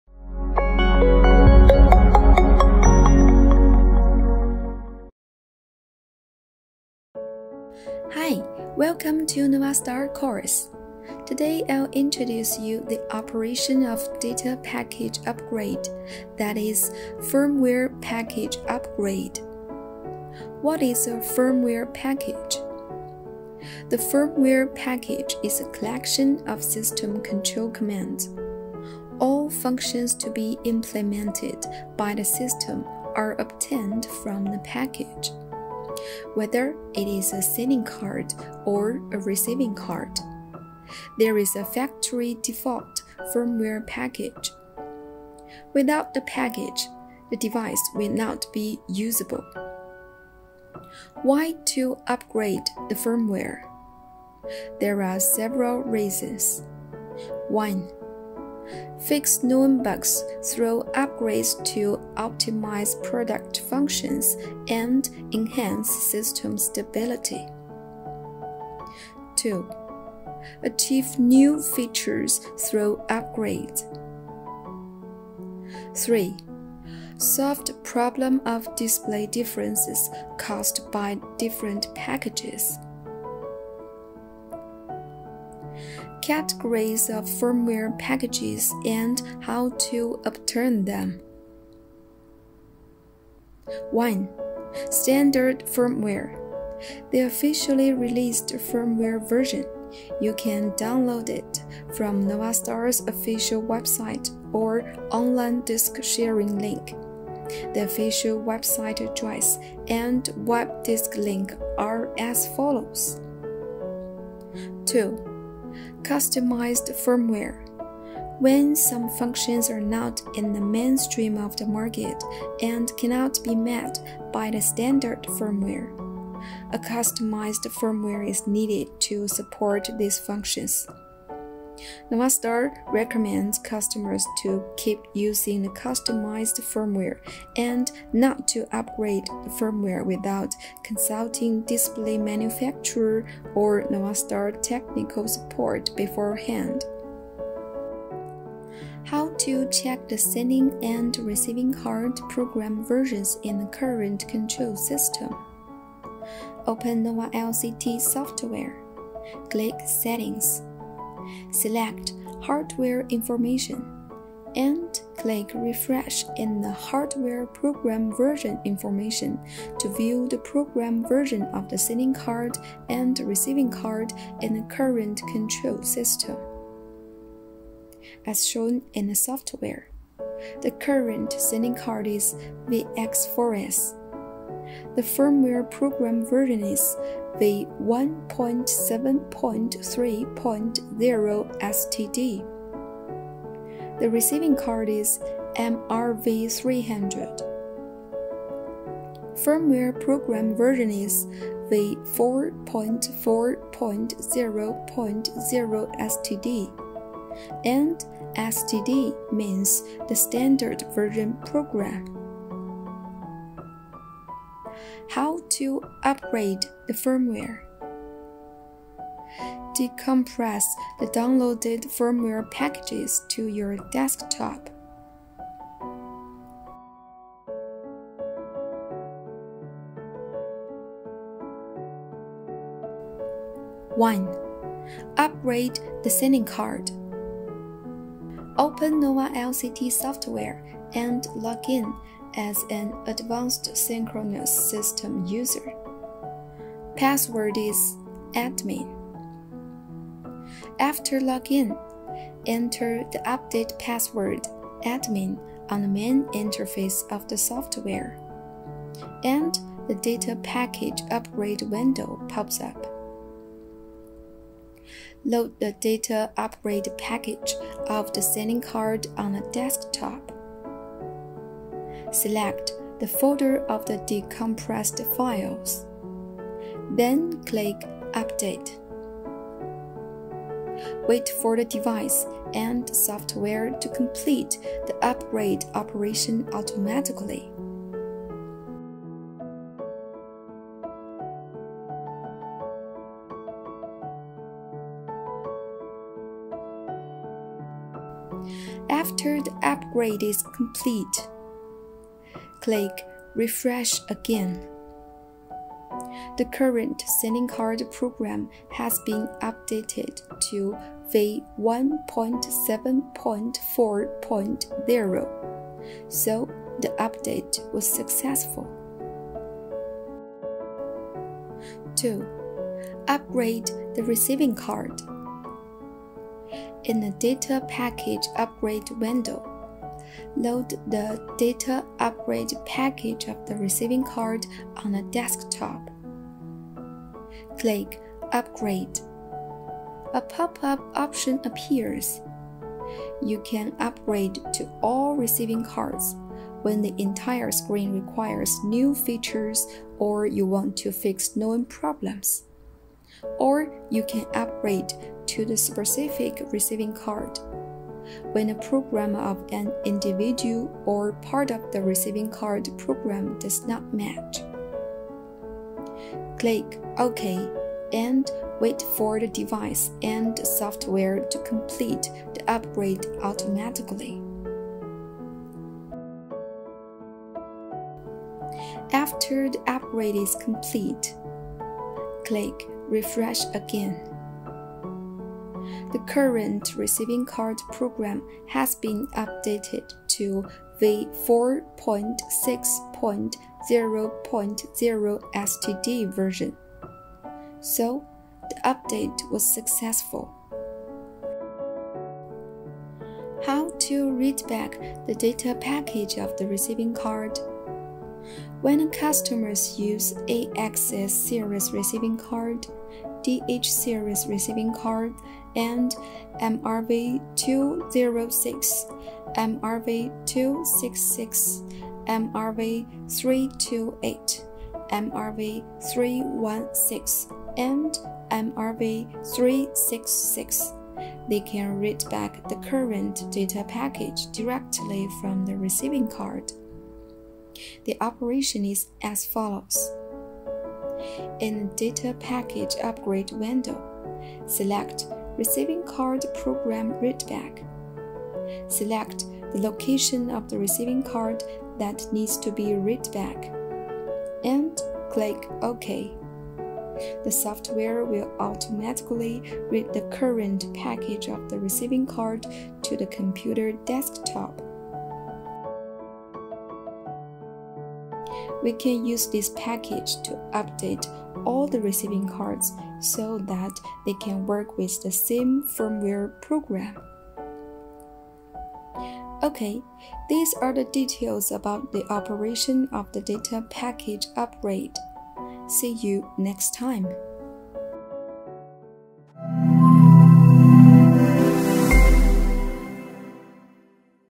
10 Firmware upgrade
Check out this video to understand how to check firmware of your controller and receiving cards, and when & how to upgrade them if necessary.